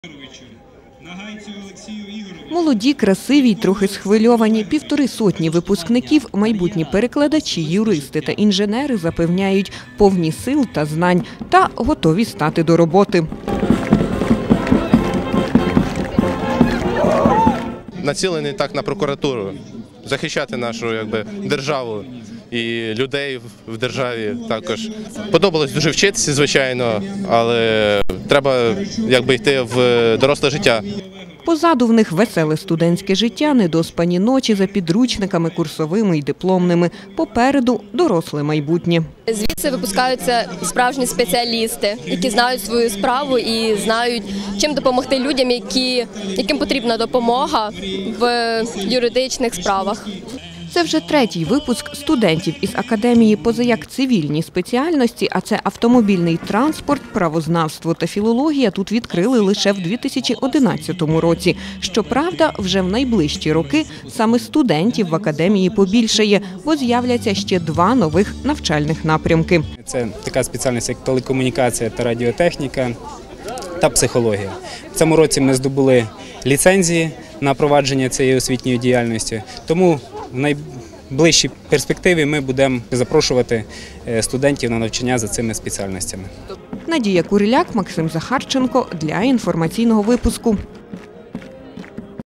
Вперед у доросле життя
В останній день червня в Національній академії  Державної прикордонної служби України імені Богдана Хмельницького випустила 158 молодих фахівців. Цивільним студентам, а це – майбутні  перекладачі, юристи та інженери –  вручили дипломи бакалаврів та спеціалістів.